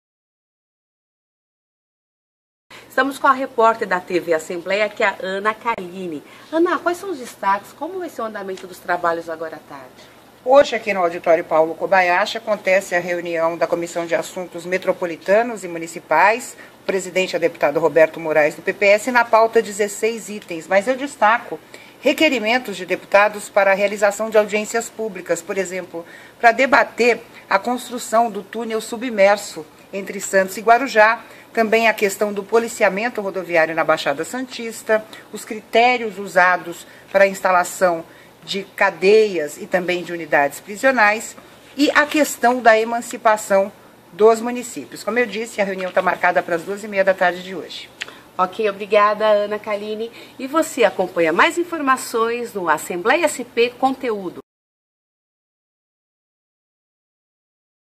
Comissão de Assuntos Metropolitanos e Municipais
A repórter Ana Kalyne destaca quais serão os assuntos da reunião de Comissão de Assuntos Metropolitanos e Municipais, no Auditório Paulo Kobayashi, na Assembleia Legislativa.